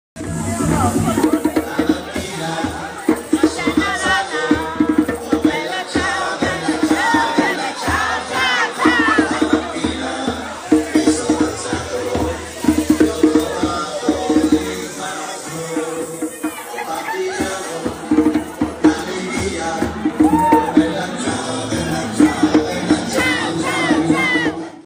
The Roaming Drummer Live - AliveNetwork.com
Bringing the action straight to your guests, The Roaming Drummer will liven up your dance floor with his incredible beats, hopping between tables, or roaming throughout your drink’s reception getting everyone involved! Previous clients and performances include Tom Jones, Hedkandi, Glastonbury Festival, Kensington Roof Gardens, Sophie Ellis Bexter and many more!